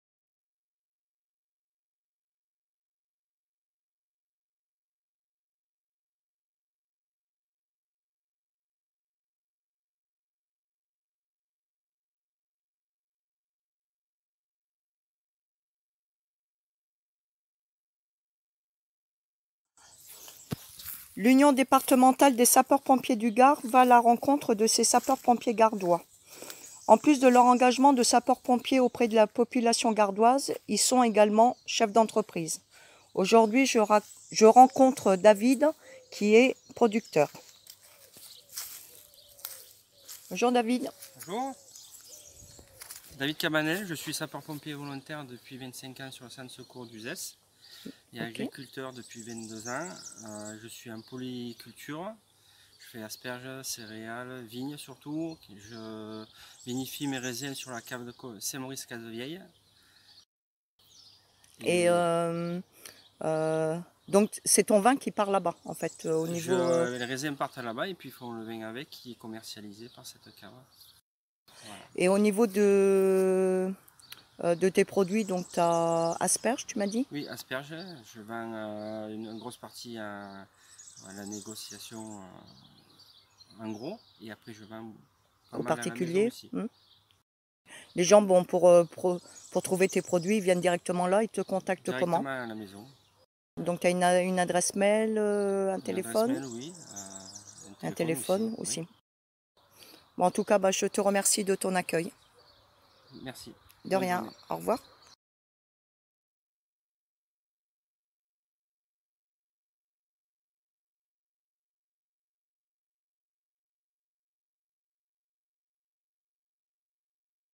CABANEL DAVID, SPV au CIS Uzès
L'Union Départementale des Sapeurs-Pompiers du Gard à la rencontre de Nos SPV Gardois Chefs d'entreprise.Ils ont tous un point commun, en plus de leur engagement de Sapeur-Pompier pour porter secours à la population, ils sont soit artisan, commerçant, profession libérale, agriculteur, en autoentreprise, en société, en nom propre
Retrouvons David CABANEL, Sapeur-Pompier Volontaire au Centre de Secours d'Uzès